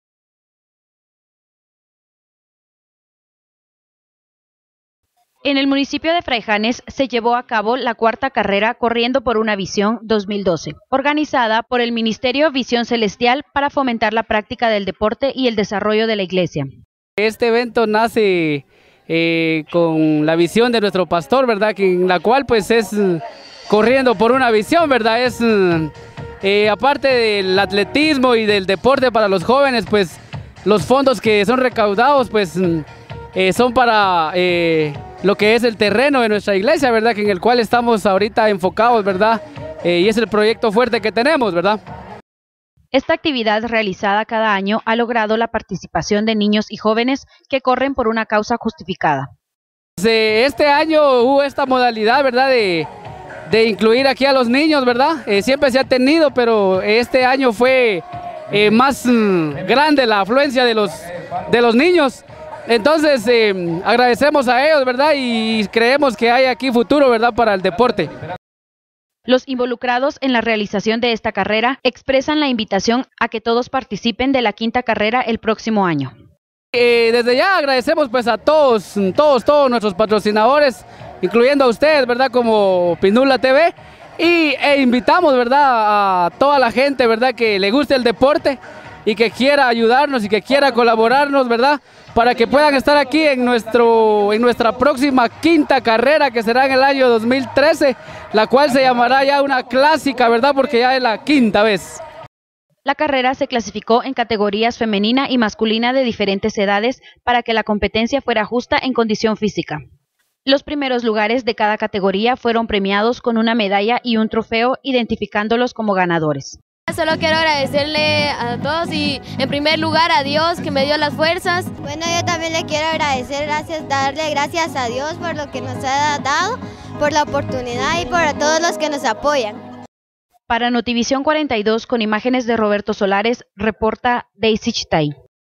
1980 Cuarta Carrera Por Una Visión.avi
En el municipio de Fraijanes se realizó la cuarta edición de la carrera denominada "Corriendo Por Una Visión" organizada por el Ministerio Visión Celestial, para fomentar la práctica del deporte.